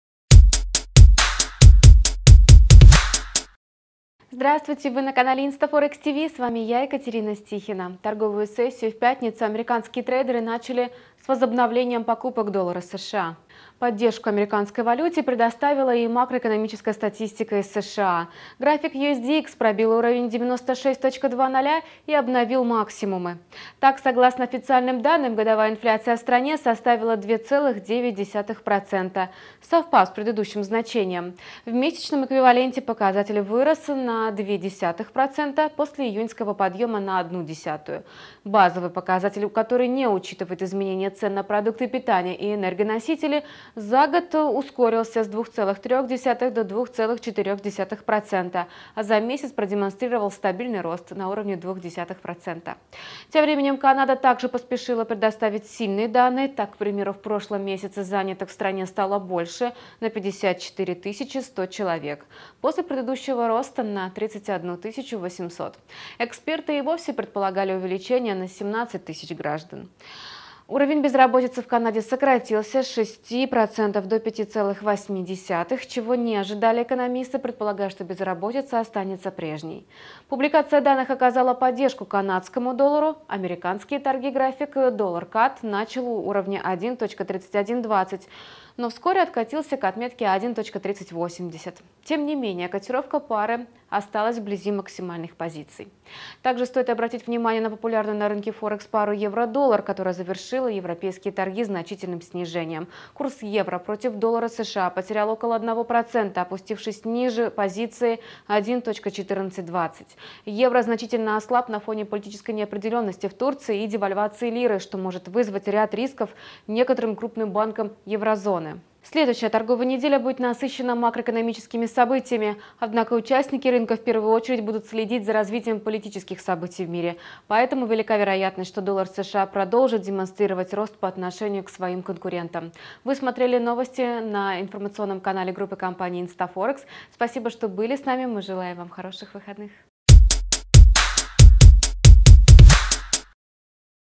Доллар США на максимуме, несмотря на слабый отчет по инфляции (10.08.2018)
Торговую сессию в пятницу американские трейдеры начали с возобновлением покупок доллара США.
Поддержку американской валюте предоставила и макроэкономическая статистика из США. График USDX пробил уровень 96.00 и обновил максимумы.
Так, согласно официальным данным, годовая инфляция в стране составила 2,9%, совпав с предыдущим значением. В месячном эквиваленте показатель вырос на 0,2% после июньского подъема на 0,1%. Базовый показатель, который не учитывает изменение цен на продукты питания и энергоносители, за год увеличился с 2,3% до 2,4%, а за месяц продемонстрировал стабильный рост на уровне 0,2%.
Тем временем, Канада также поспешила предоставить сильные данные. Так, к примеру, в прошлом месяце занятых в стране стало больше на 54 100 человек, после предыдущего роста на 31 800. Эксперты и вовсе предполагали увеличение на 17 000 граждан.
Уровень безработицы в Канаде сократился с 6,0% до 5,8%, чего не ожидали экономисты, предполагая, что безработица останется прежней.
Публикация данных оказала поддержку канадскому доллару. Американские торги график USD/CAD начал у уровня 1.3120 и вскоре откатился к отметке 1.3080. Тем не менее котировка пары осталась близи максимальных позиций.
Также стоит обратить внимание на популярную на рынке Форекс пару, которая завершила европейские торги значительным снижением. Курс евро против доллара США потерял около 1% , опустившись ниже позиции 1.1420.  Евро значительно ослаб на фоне политической неопределенности в Турции и девальвации лиры, что может вызвать ряд рисков некоторым крупным банкам еврозоны.
Следующая торговая неделя будет насыщена макроэкономическими событиями, однако участники рынка в первую очередь будут следить за развитием политических событий в мире. Поэтому, велика вероятность, что доллар США продолжит демонстрировать рост по отношению к своим конкурентам.